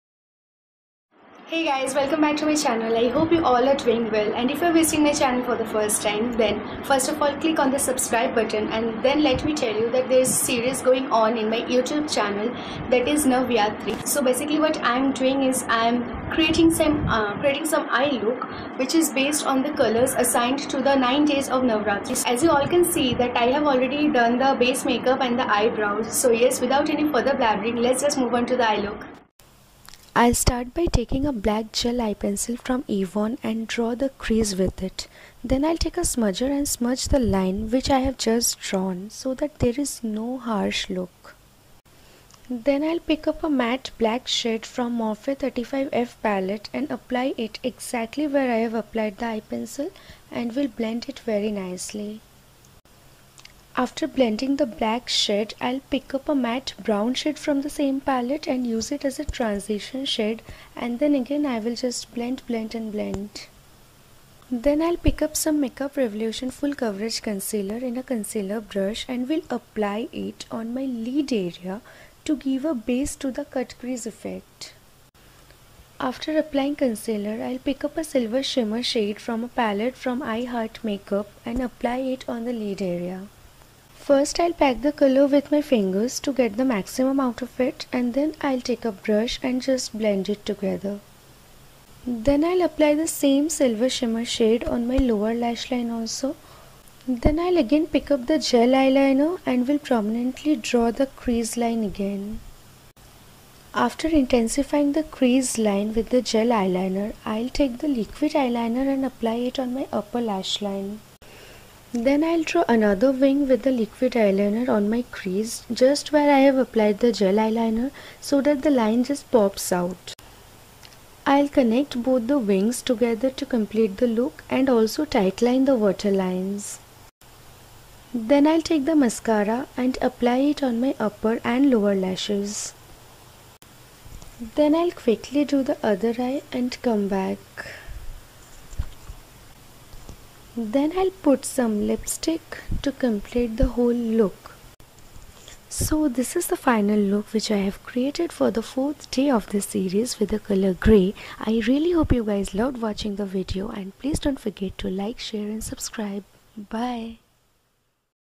NavEYEtri Series| Grey Eyemakeup|Graphic eyeliner|Silver eyes|Navratri makeup|Sohini Bhattacharya
Hey guys! This is the fourth video of the series "navEYEtri"

This series is all about a eye makeup looks for 9 days of Navratri which will be coordinated with the colors for each day of Navratri. Though you can try this looks on any other occasion too

I hope you all like the video and will enjoy the series "navEYEtri"

Be yourself be beYOUtiful
Love
Sohini